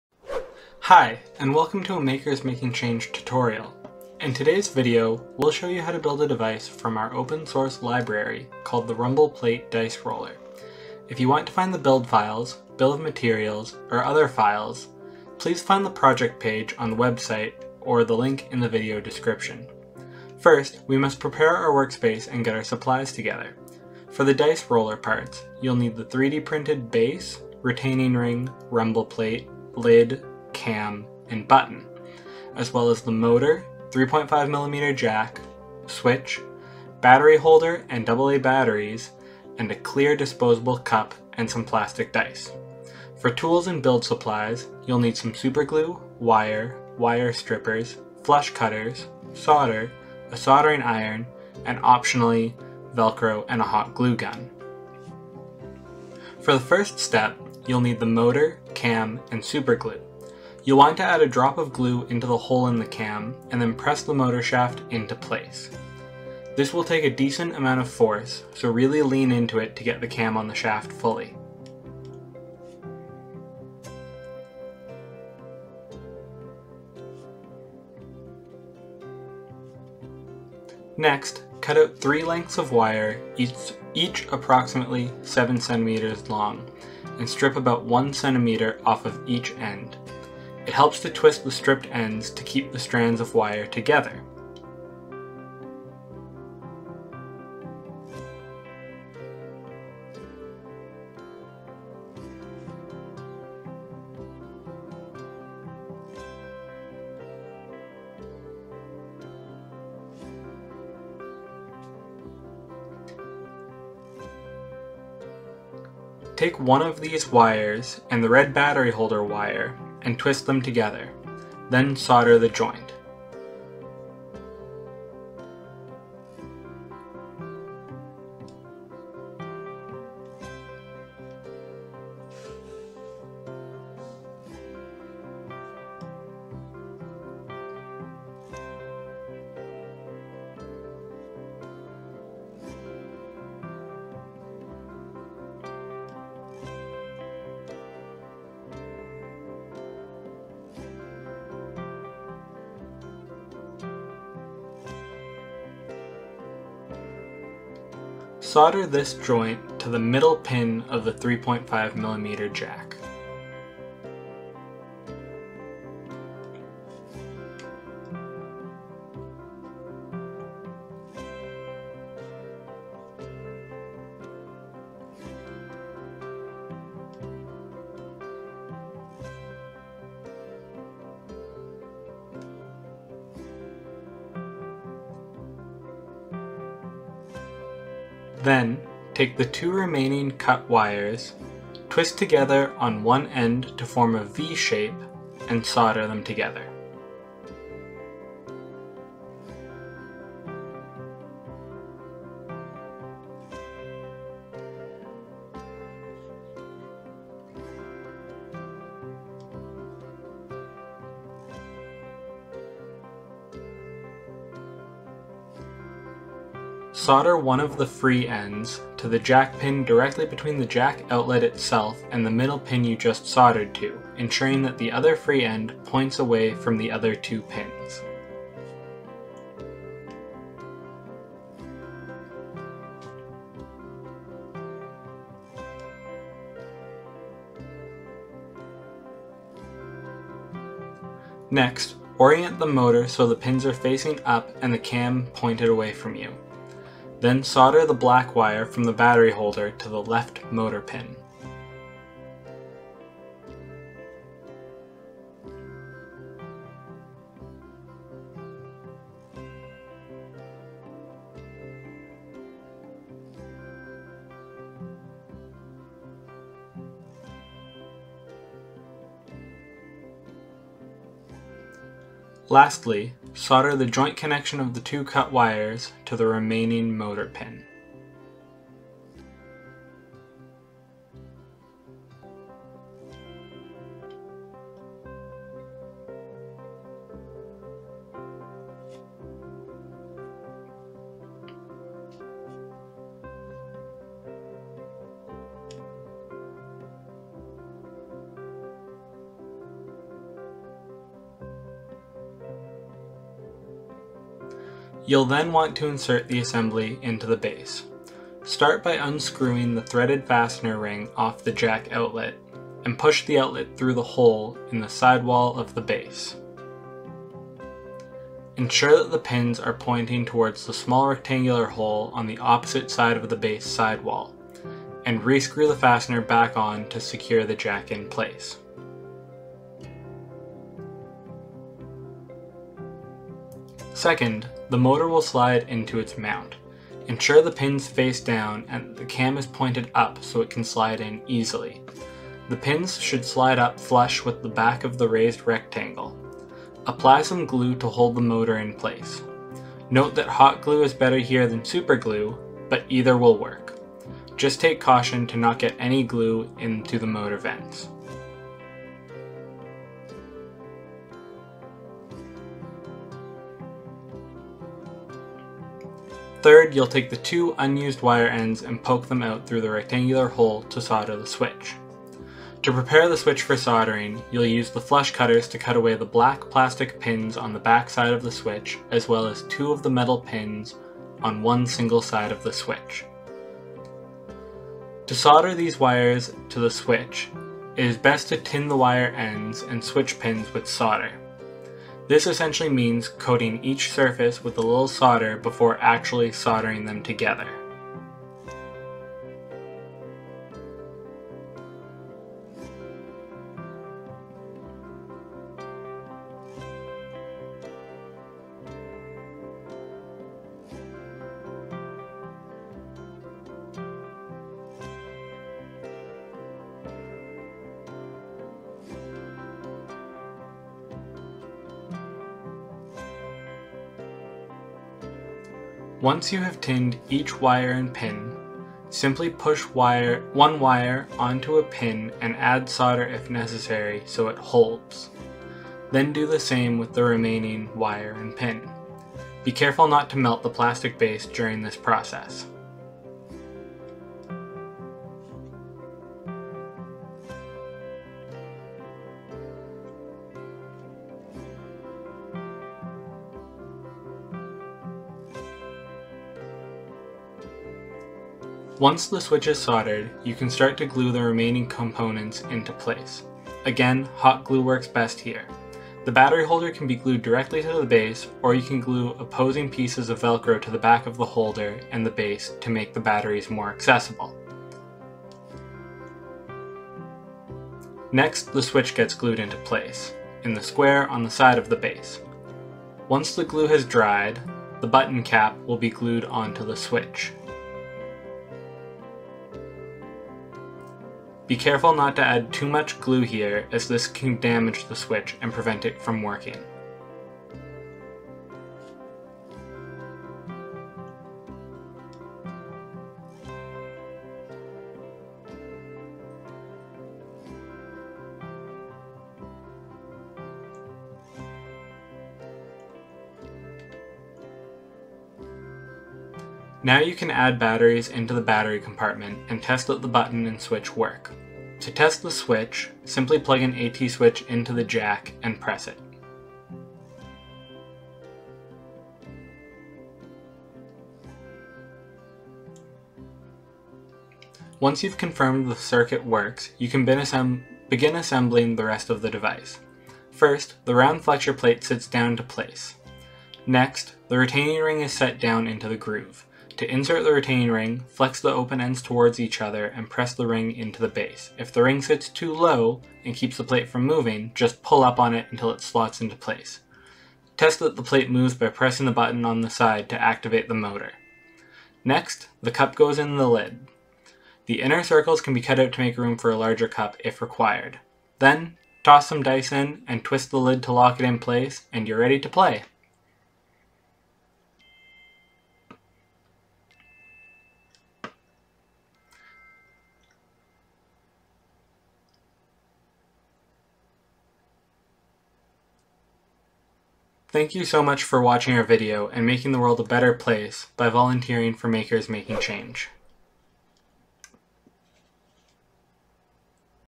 Dice Roller Build Tutorial
The Rumble Plate Dice Roller is a switch activated device that makes any dice game more accessible.

An assistive switch switch can be plugged into the 3.5mm jack on the side of the device to be used for activation. Alternatively, the on-device switch can be used for activation, too.

Volunteer with Makers Making Change to build this switch and other assistive devices for people with disabilities.